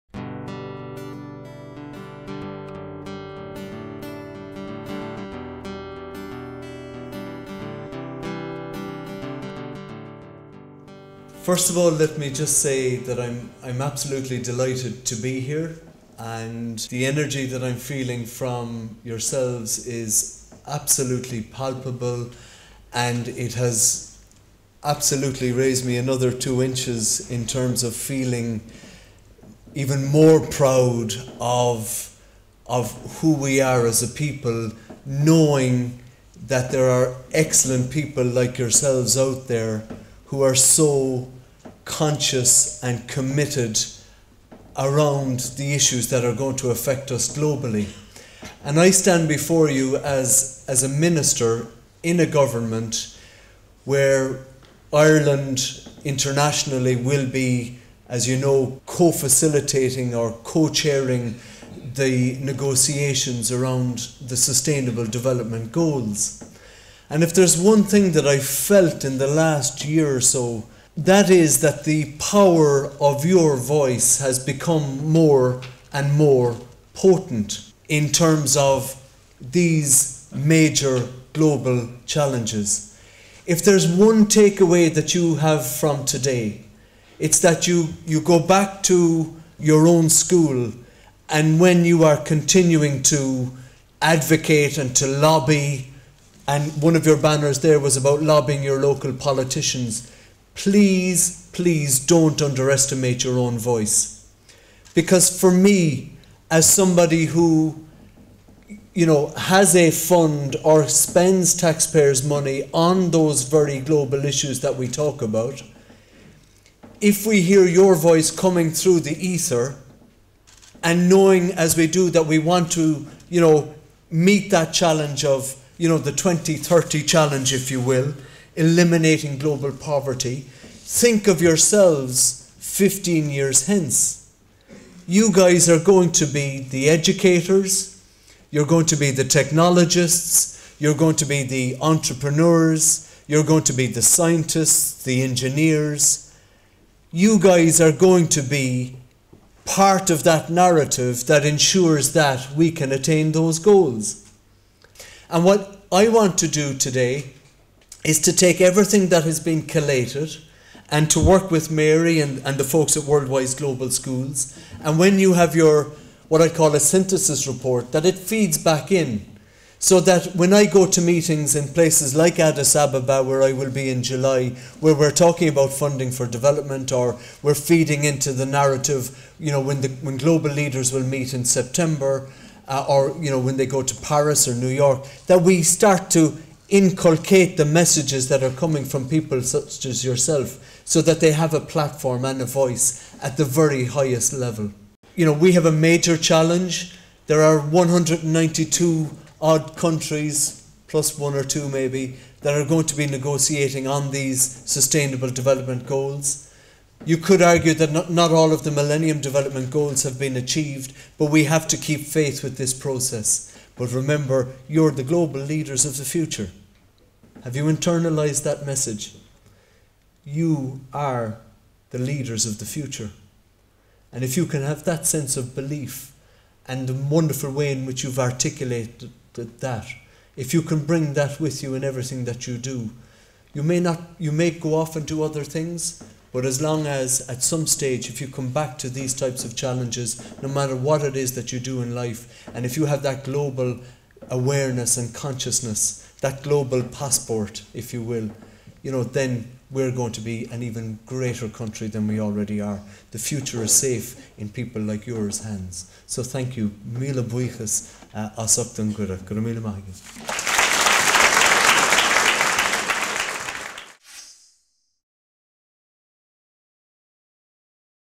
Worldwise Conference 2015: Closing Speech from Minister Sean Sherlock, TD
A short summary of the main points raised by Minister of State Sean Sherlock during his closing remarks at the Worldwise Global Schools Conference in the Aviva Stadium, April 21st 2015.